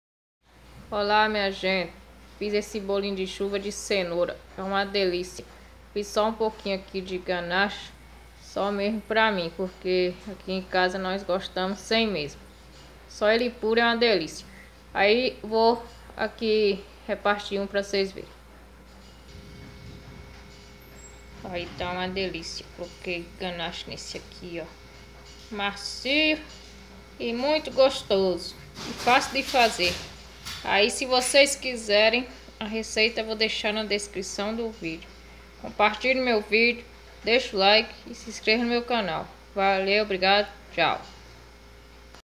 Receitas da Marla(Bolinho de chuva de cenoura 😋😋).
Ingredientes:1 cenoura média ralada,2 ovos,3 xícaras de farinha de trigo,1 xícara de açúcar,2 xícaras de leite,1 colher de sopa de fermento em pó.Modo de fazer:Bate todos ingredientes no lidificador menos a farinha e fermento,depois add a mistura do lidificador na farinha mistura bem e add o fermento,frita no óleo não muito quente e fogo baixo,depois pode comer com brigadeiro ou ganache .Pronto é só saborear😋